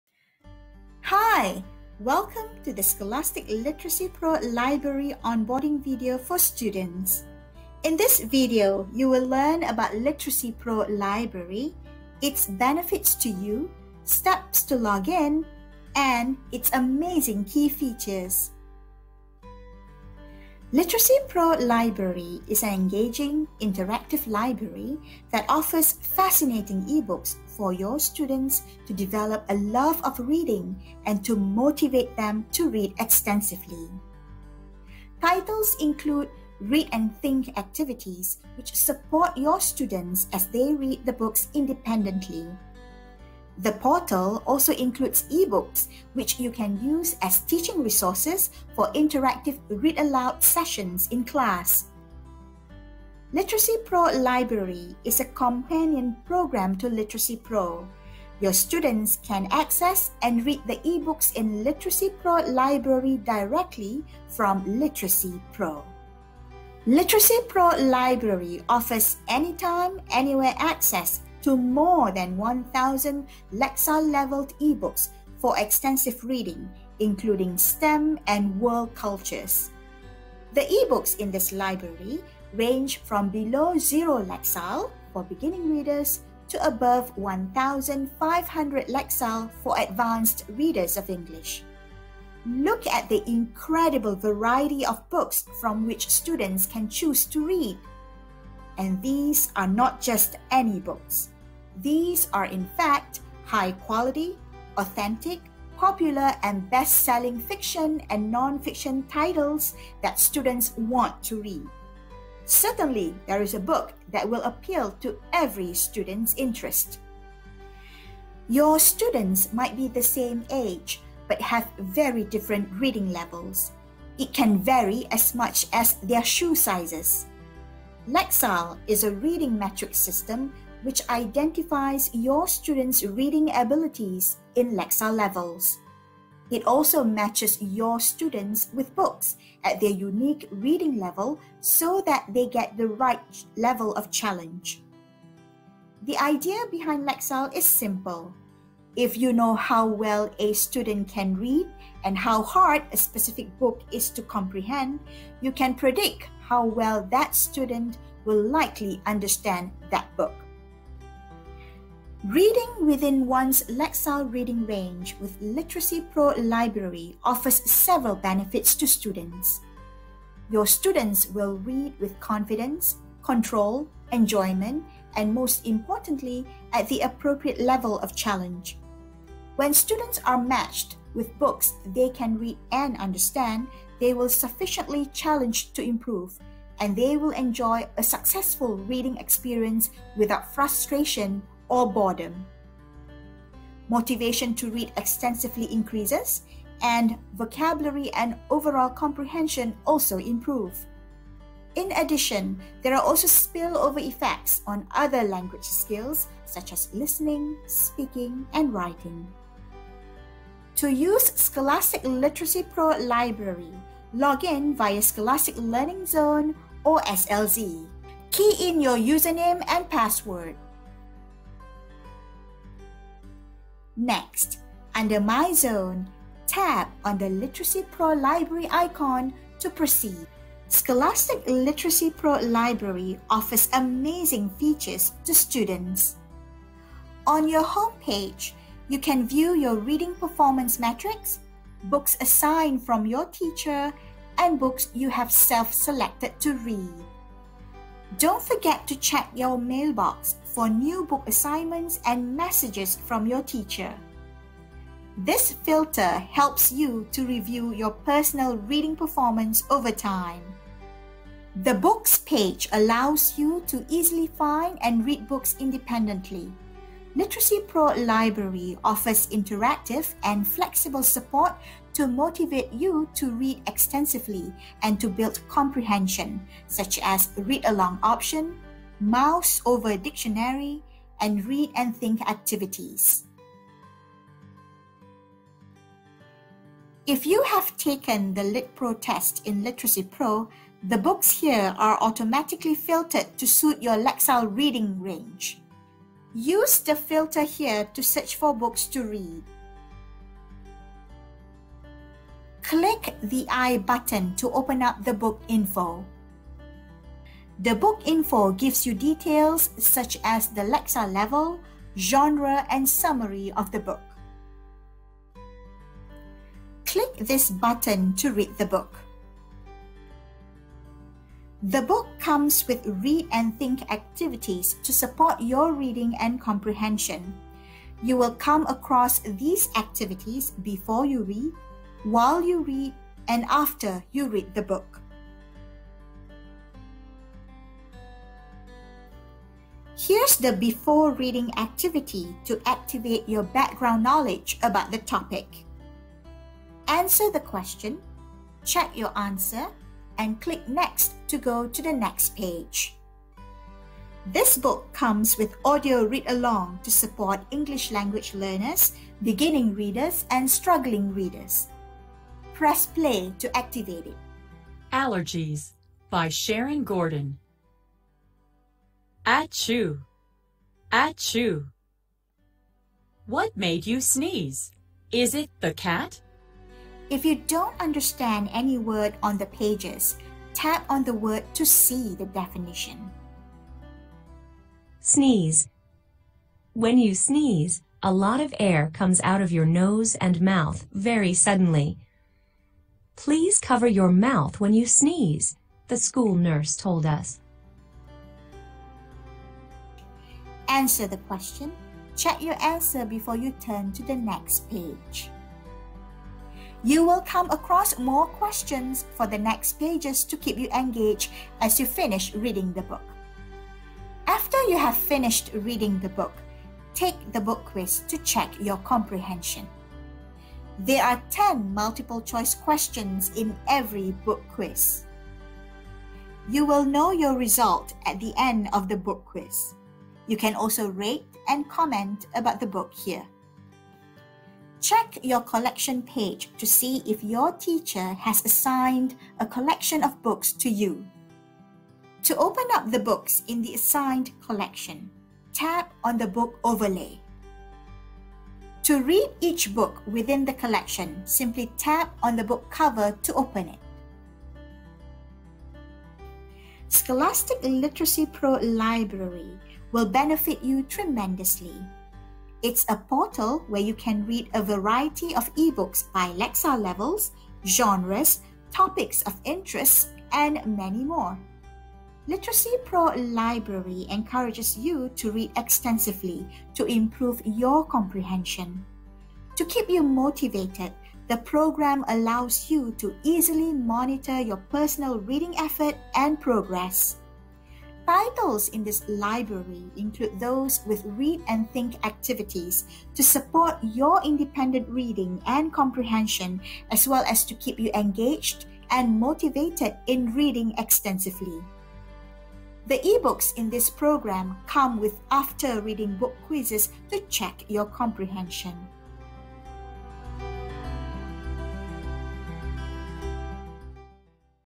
Scholastic Literacy Pro Library: Transforming Students into Confident Readers in School and at Home
Tutorial for Students

Let's Get Started! Learn about how Literacy Pro Library will motivate and excite children to read the right books for their reading level and interests!

0:00 What is LPL?
1:06 Overview: Product content
1:58 Brief understanding of Lexile measures
2:41 Benefits of Reading in Lexile
3:30 Steps to log-in as a Student (Demo starts)
5:42 Key Features (featuring Read & Think activities)
7:37 Brief understanding and steps to use LP Reading Comprehension Skills